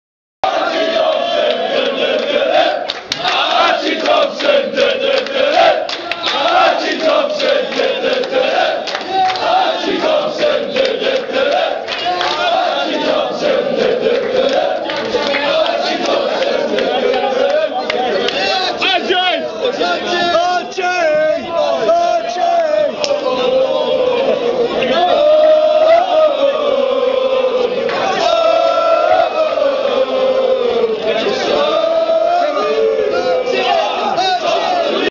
Archie Thompson De De De De
Archie Thompson makes a suprise appearance as he walks through a mob of Melbourne fans at the function after Melbourne Victory beat Adelaide at Hidnmarsh 2007